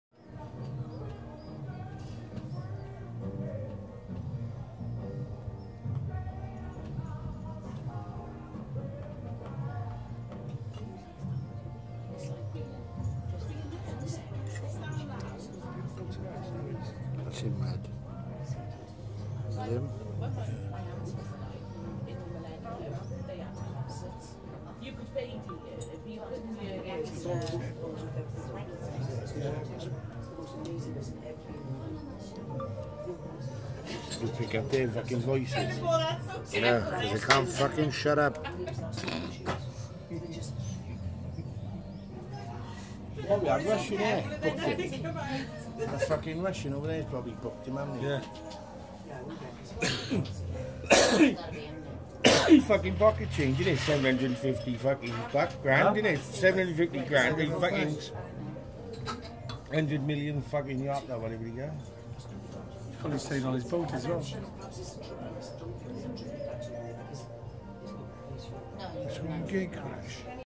Elton John - Sings for Avoth in Monte Carlo
Elton John - Singing in Monte Carlo Sporting Club for Eddie Avoth